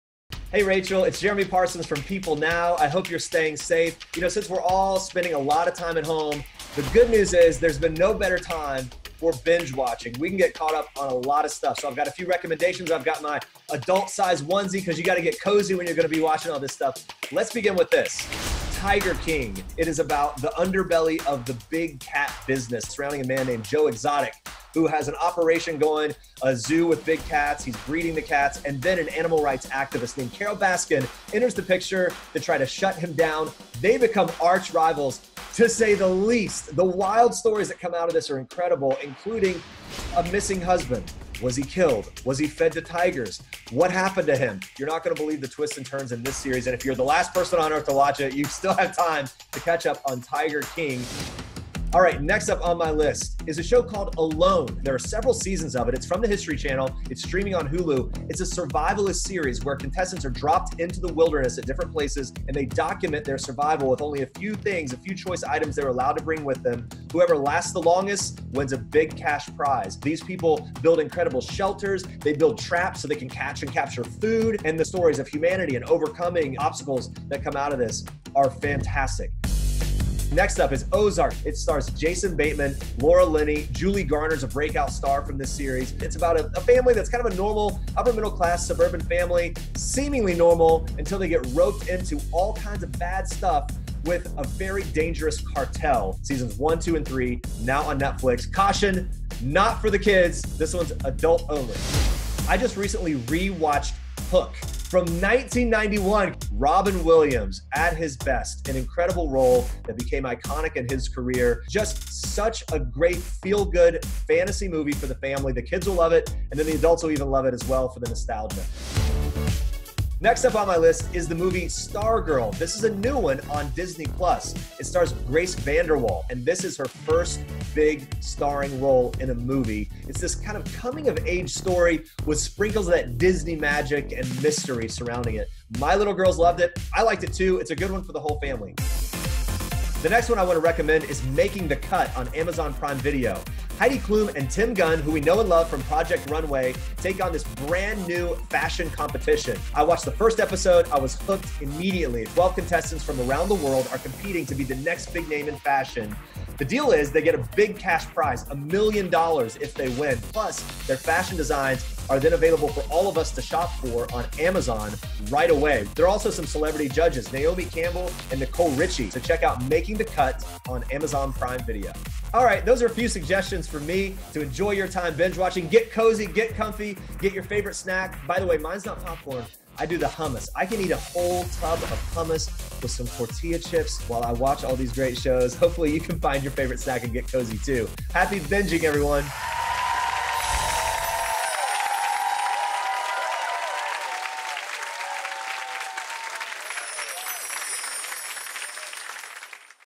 What To Binge-Watch While You StayHome | People Now Host Jeremy Parsons
The best TV shows & movies to binge-watch while social distancing—family-friendly options, picks for adults + new series—from "People" host Jeremy Parsons.